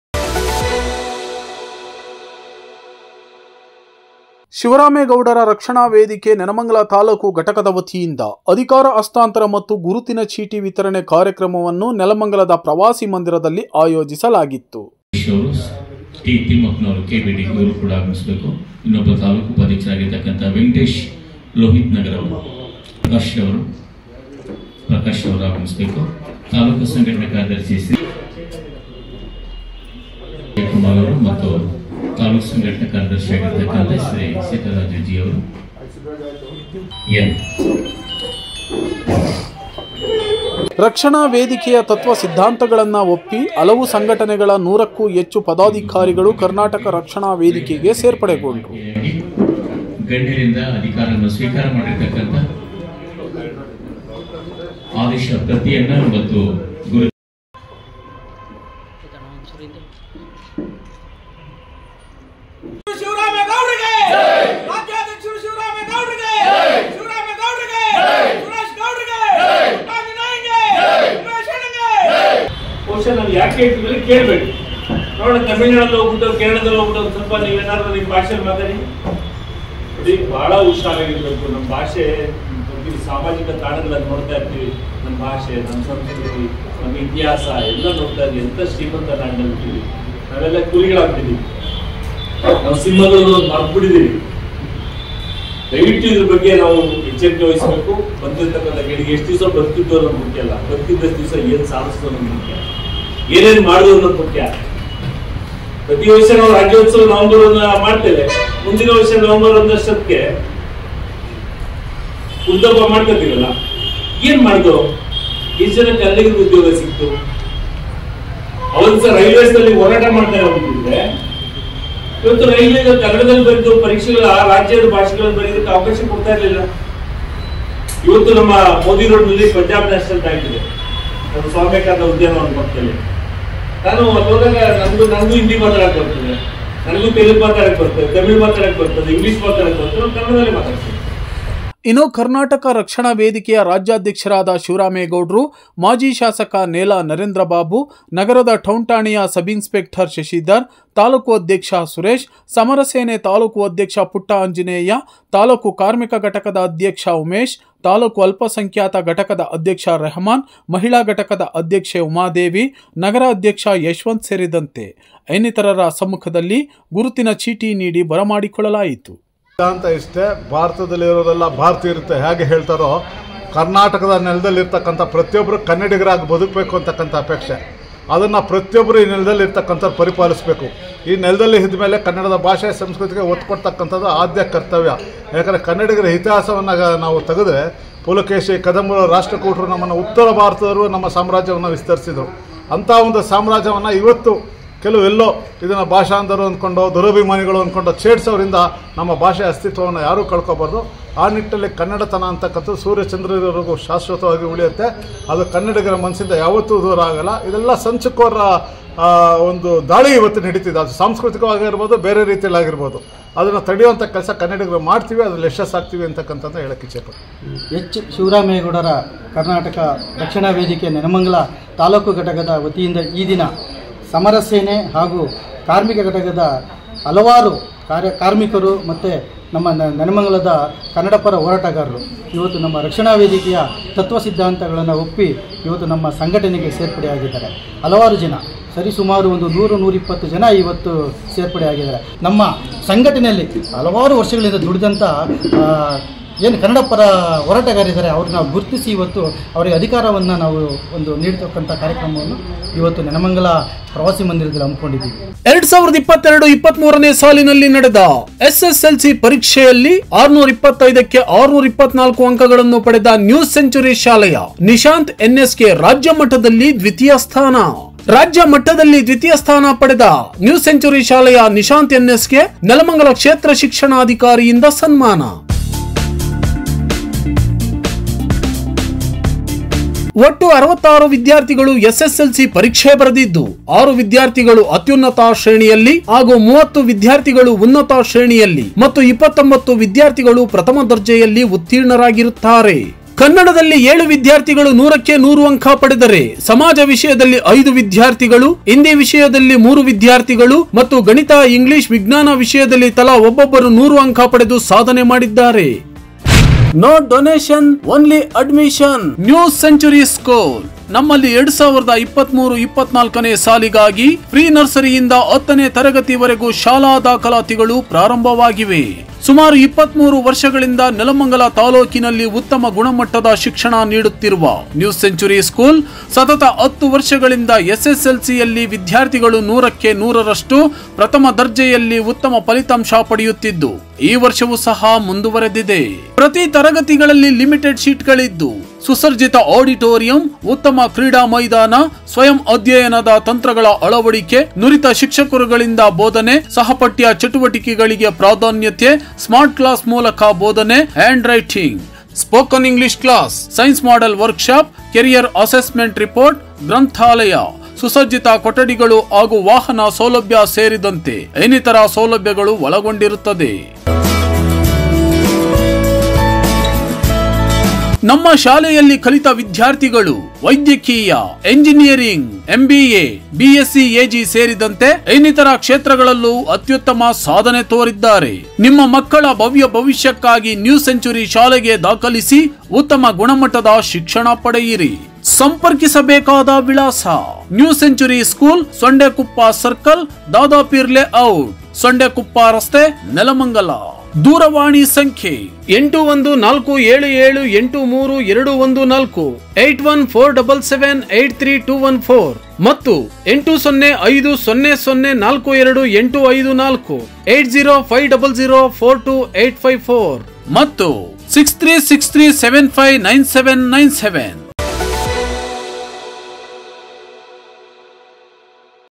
ಕರ್ನಾಟಕ ರಕ್ಷಣಾ ವೇದಿಕೆಗೆ ಕಾರ್ಯಕರ್ತರ ಸೇರ್ಪಡೆ,ಅಧಿಕಾರ ಹಸ್ತಾಂತರ ಮತ್ತು ಗುರುತಿನ ಚೀಟಿ ವಿತರಣೆ.
ವಾಗ್ಮಿ ಕನ್ನಡ/vagmi kannada